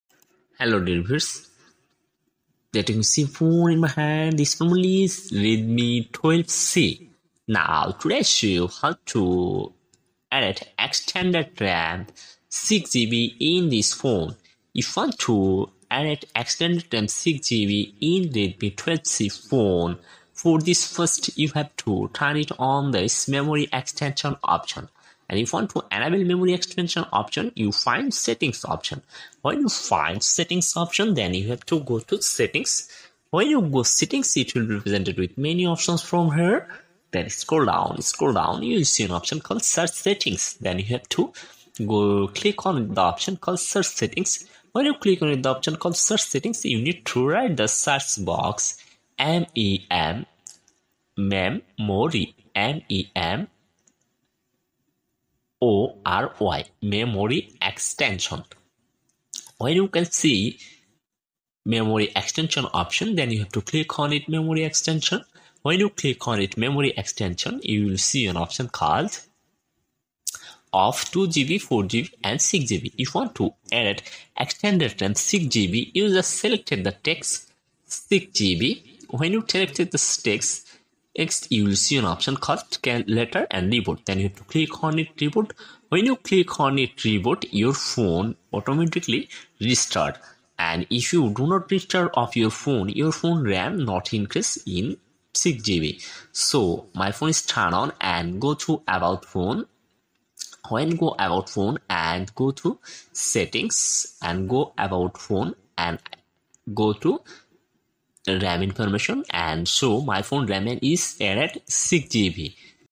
Redmi 12c Memory extension //how to increase ram in redmi 12c
Now today i will show you how to enable ram extension in redmi 12c phone.if you want to enable ram extension in this phone go to settings and enable your phone ram extension option.if you want to enable ram extension go settings and increase ram 6gb 4gb or 2gb you just select the text and restart your phone and increase your phone ram.